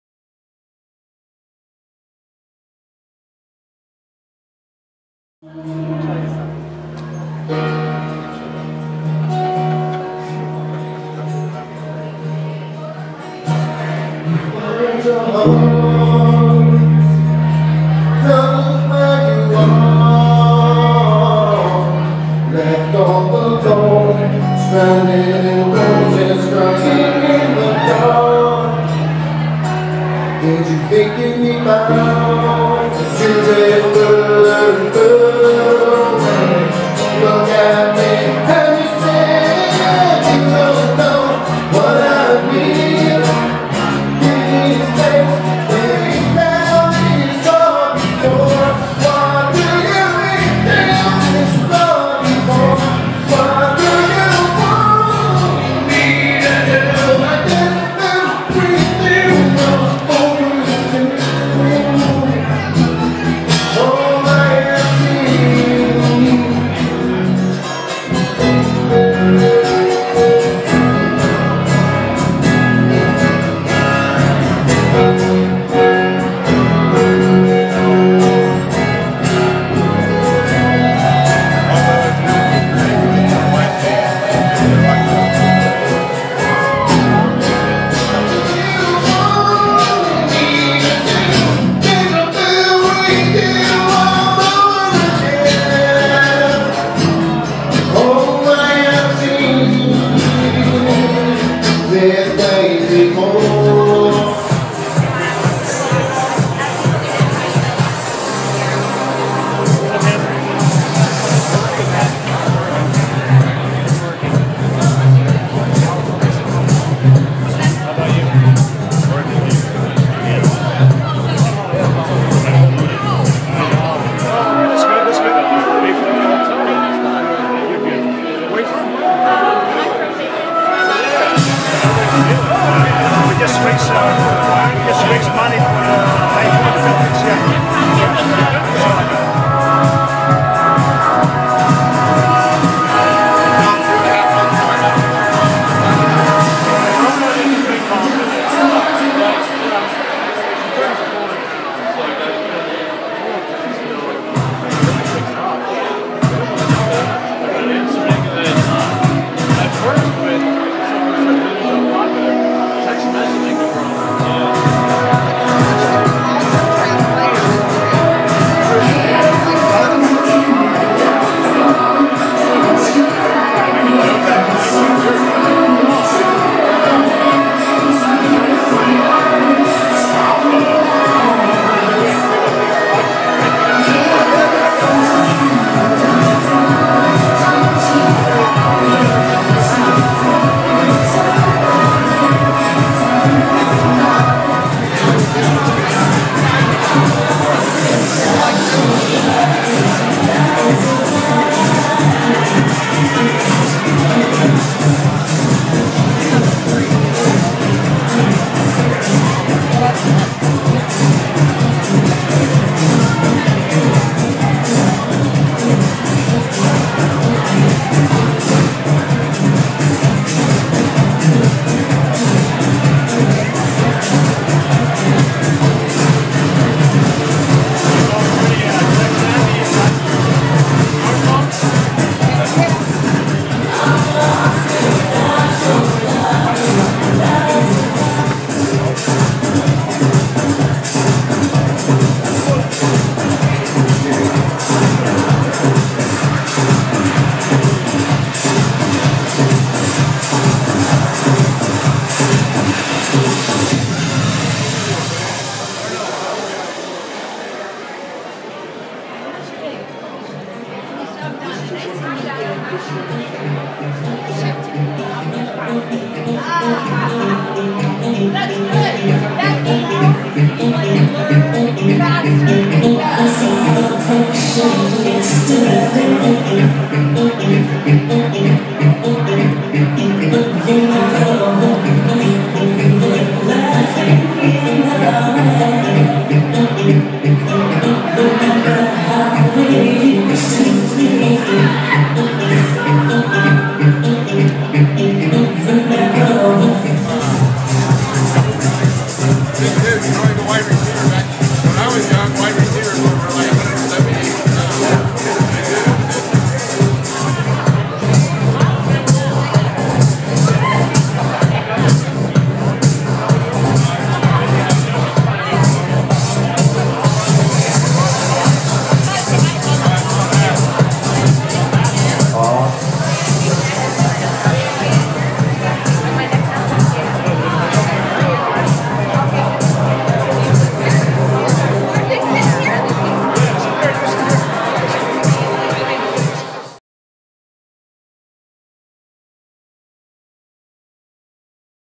Karley Cottrell aerial silks & lyra performance
Aerial Silks and Lyra performance at SHē nightclub for the "Night of Angels" benefit and fundraiser concert. Las Vegas, NV 12-3-13